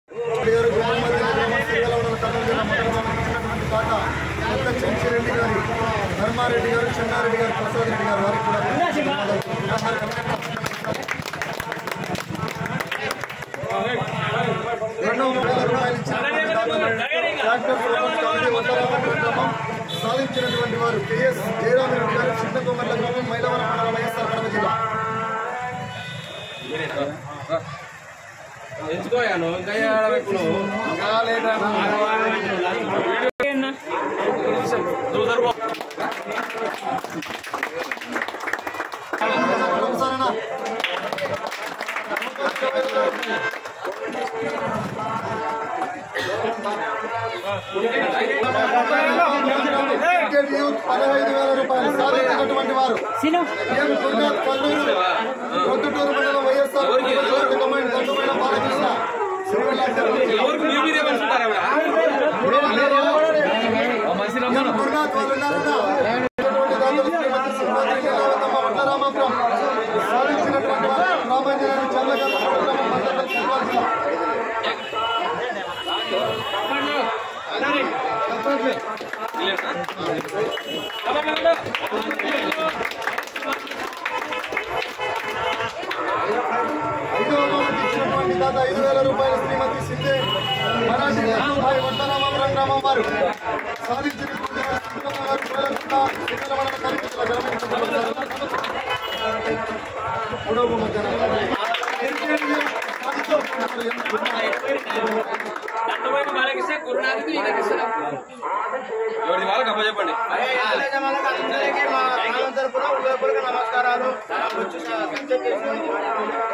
కర్నూలు జిల్లా వడ్ల రామాపురం ఆత్మకూరు (మ) గ్రా న్యూ క్యాటగిరి బహుమతులు 1 నుండి 5
కర్నూలు జిల్లా ఆత్మకూరు మండలం వడ్ల రామాపురం గ్రామం న్యూ క్యాటగిరి బహుమతులు సాధించినవారు 1 నుండి 5🏆🥇🥈🥉🏆